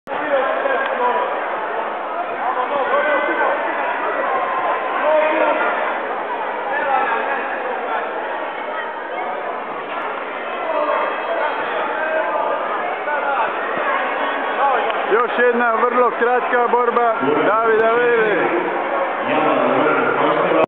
JK Biokovo - David Vela
Davidov brzi ippon br. 2. Sv. Duje, Split, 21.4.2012.